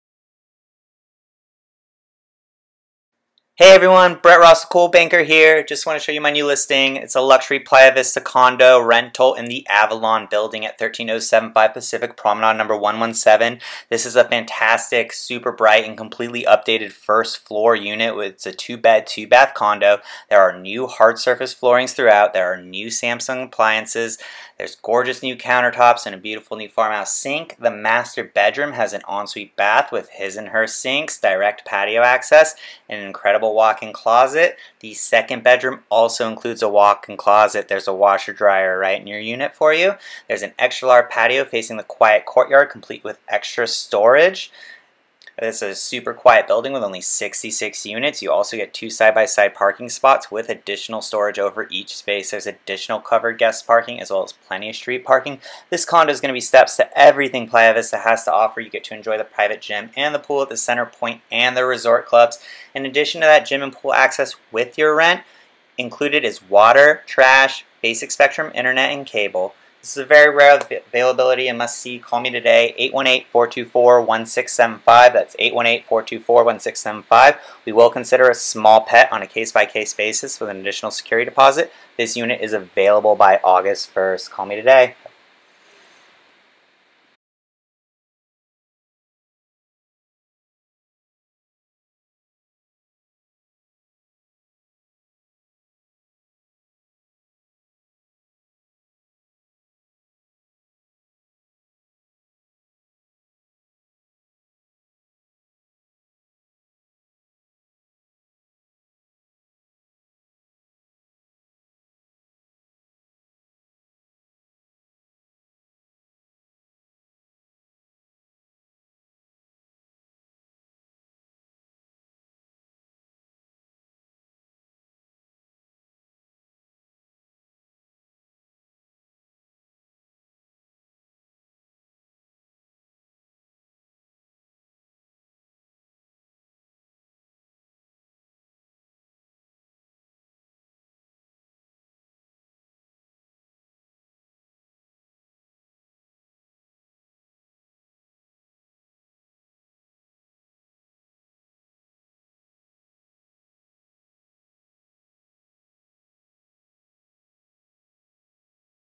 For Rent! 13075 Pacific Promenade #117 Playa Vista - MLS Video
Luxury Playa Vista living in the Avalon building! Fantastic, bright & completely updated first floor 2 bed, 2 bath condo with new hard surface flooring throughout! New Samsung stainless steel appliances, gorgeous new kitchen countertops & beautiful farmhouse sink. Master bedroom features en-suite bath with his & her sinks, direct patio access and incredible walk in closet. Second bedroom also includes a walk in closet. Washer/dryer in unit. Extra large patio facing quiet courtyard complete with storage closet. Two side-by-side parking spaces. Both parking spaces include additional secure storage over each space! This condo is steps to everything Playa Vista has to offer! Enjoy the private gym & pool at both the Centerpointe Club & the Resort. In addition to gym / pool access; water, trash & basic Spectrum internet / cable is all included in rent! This rare availability is a must see! Small pet will be considered on a case by case basis with extra deposit. Available by August 1st.